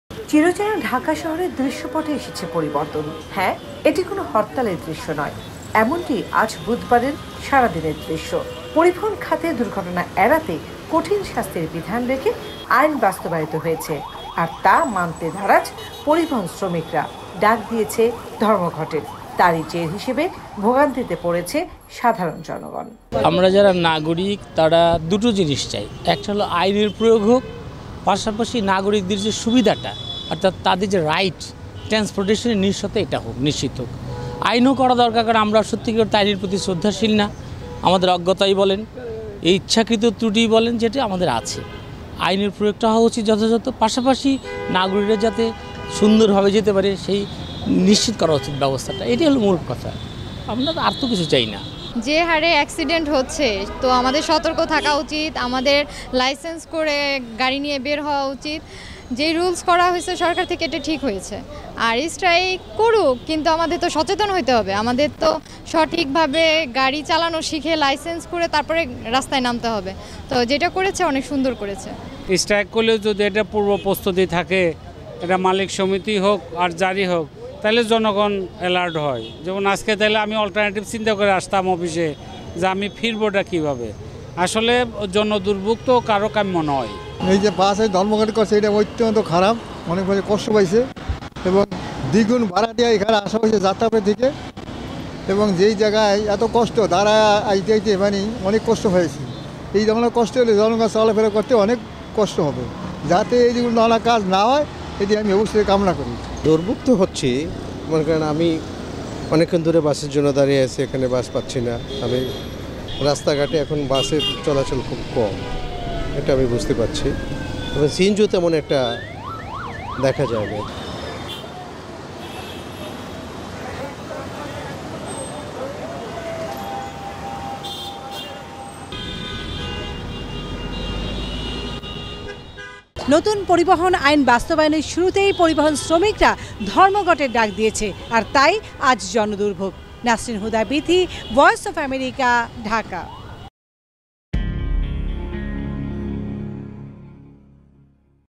Nasrin Huda Bithi / Voice of America Report / Jonodurvogh / জন দূর্ভোগ
“পরিবহন ধর্মঘটে জনজীবনে দূর্ভোগ”
চিরচেনা ঢাকা শহরের দৃশ্যপটে এসেছে পরিবতন।  হ্যাঁ এটি কোন হরতালের দৃশ্য নয়। এমনটি আজ বুধবারের সারাদিনের দৃশ্য। পরিবহন খাতে দূঘটনা এড়াতে কঠিন শাস্তির বিধান রেখে আইন বাস্তবায়িত হয়েছে, আর তা মানতে নারাজ পরিবহন শ্রমিকরা ডাক দিয়েছে ধমঘটের । তারই জের হিসেবে ভোগান্তিতে পড়েছে সাধারণ জনগণ।